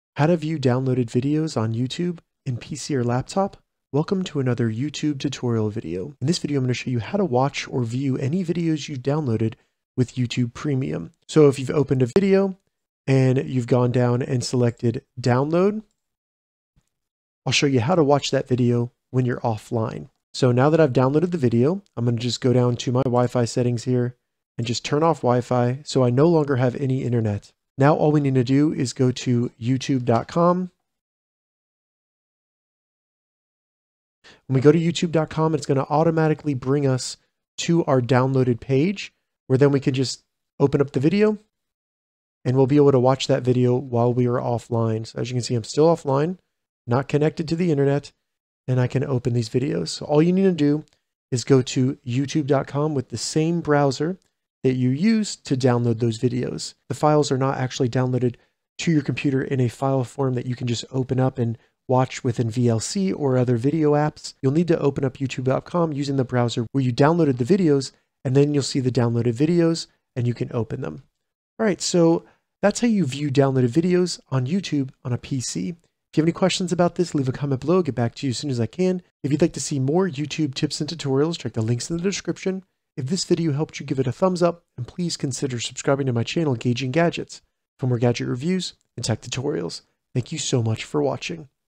How to View Downloaded Videos on Youtube in PC & Laptop
How to view downloaded videos on YouTube in PC? In this tutorial, I show you how to watch downloaded videos while offline on YouTube in PC, Laptop, Chromebook, and Macbook. This means you can find and watch any YouTube videos you download with YouTube premium. If you are offline or without internet it's easy to watch downloaded YouTube videos.